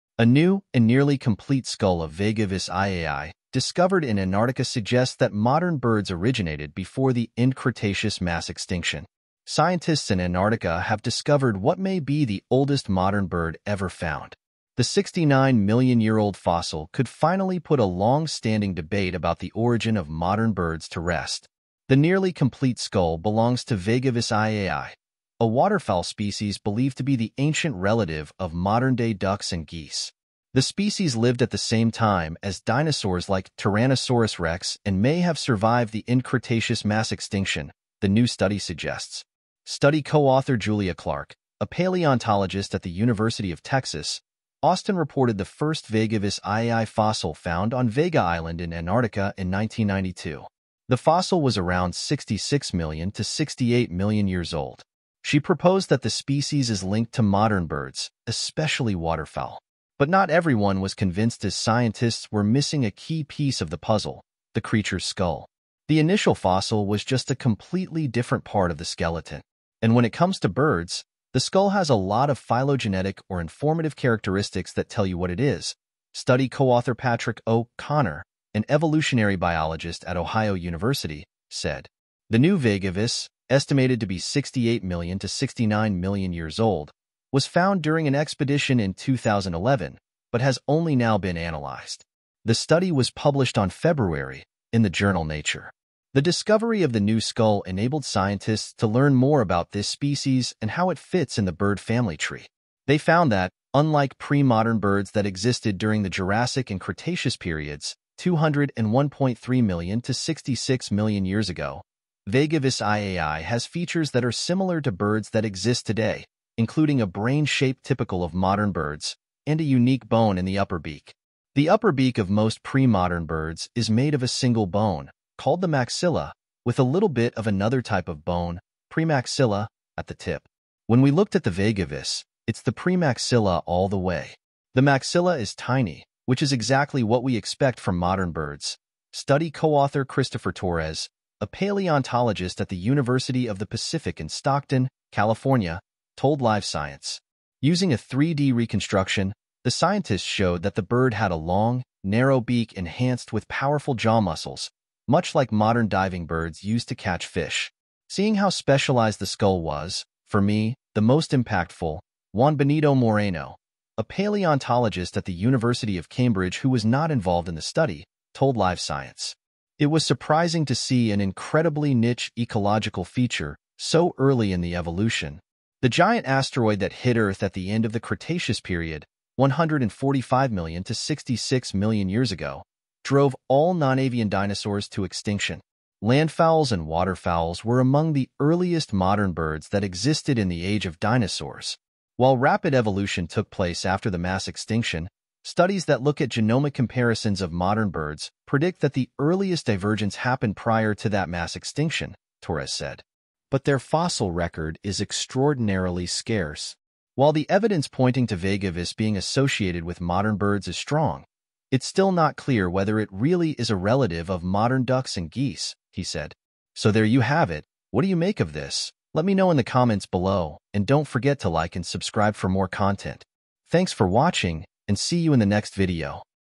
Ancient Duck Ancestor Found in Antarctica Makes Scientific History!
A new and nearly complete skull of Vegavis iaai discovered in Antarctica suggests that modern birds originated before the end-Cretaceous mass extinction.